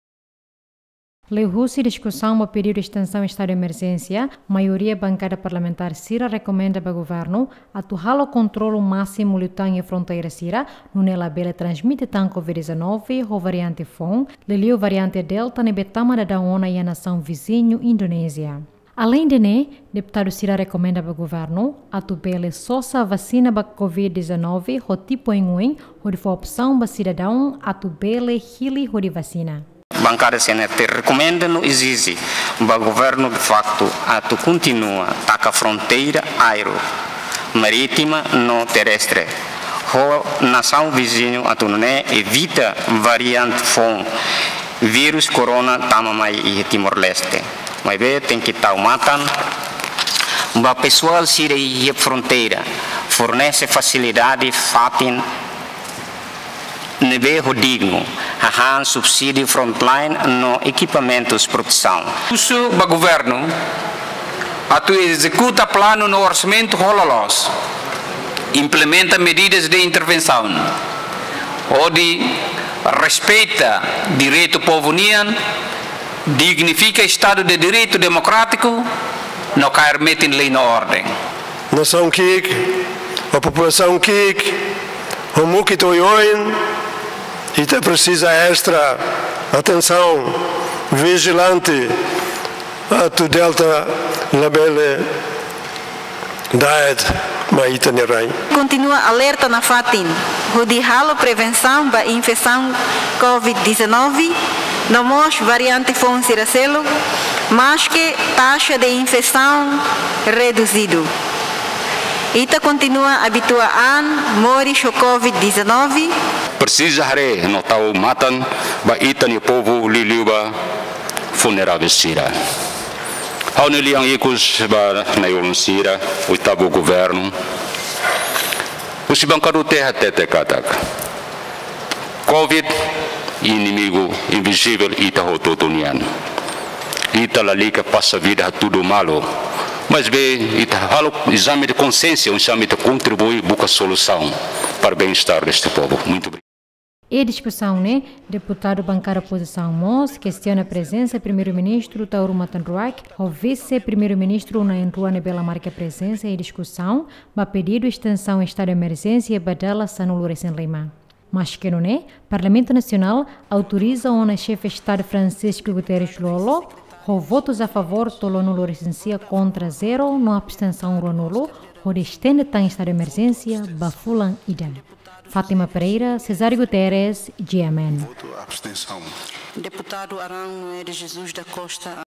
Bankada Parlamentar husu Governu reforsa kontrolu iha fronteira hodi prevene Variante Delta mai TL
Bankada Parlamentar sira husu Governu reforsa kontrolu iha fronteira hodi prevene transmisaun COVID-19 variante Delta mai TL